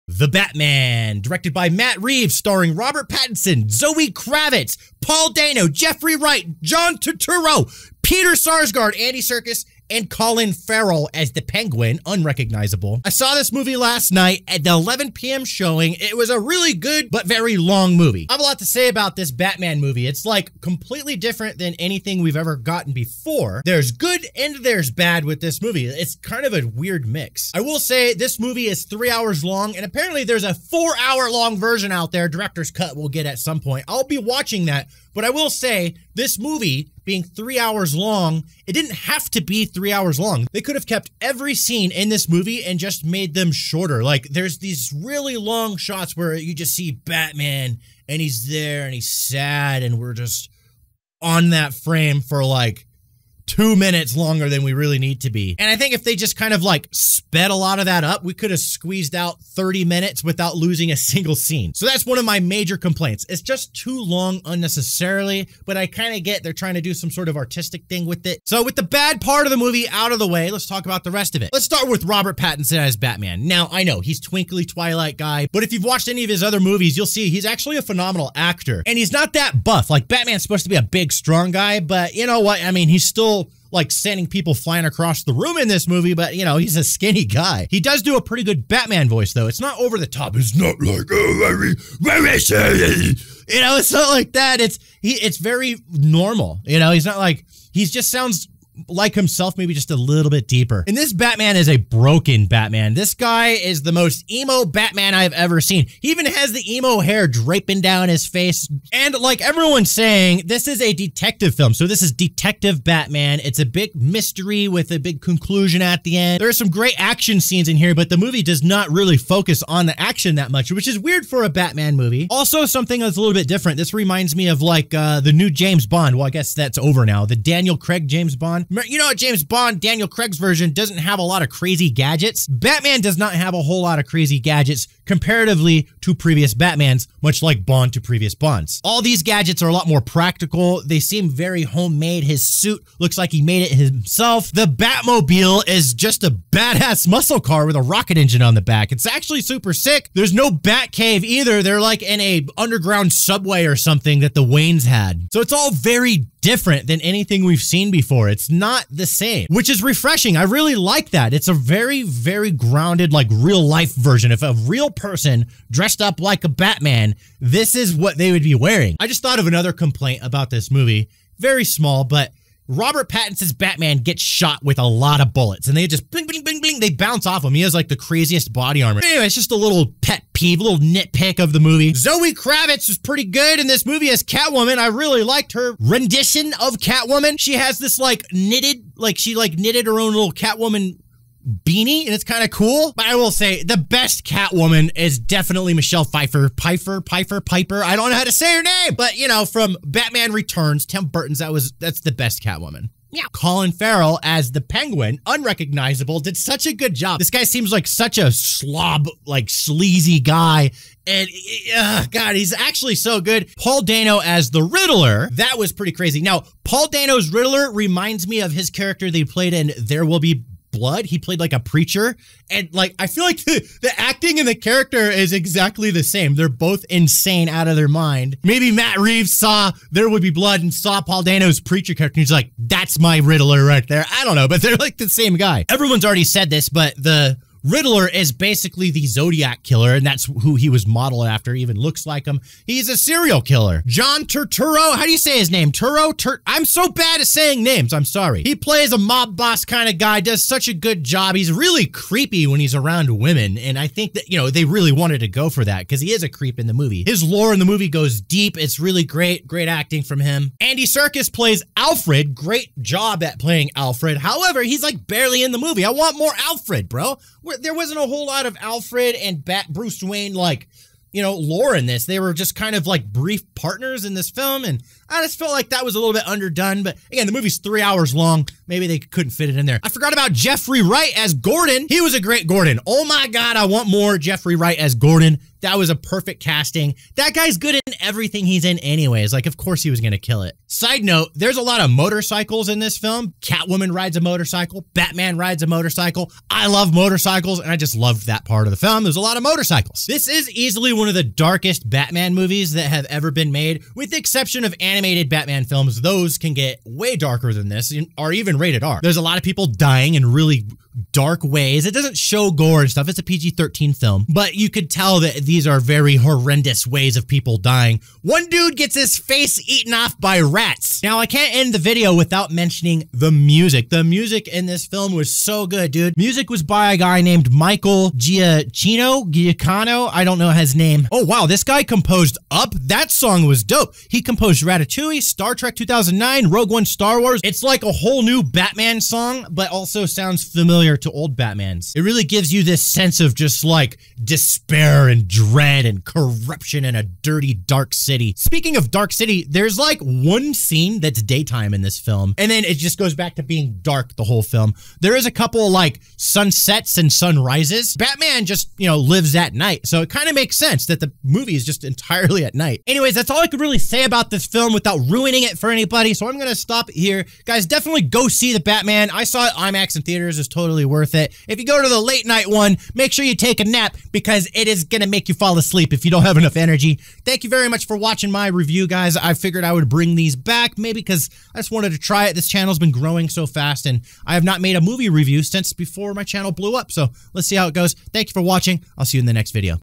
The Batman Movie Review - Papa Reviews
After a power nap I caught the 11pm showing and sat through the this 3 hour piece of art. Here are my thoughts.

12470 York St. # 638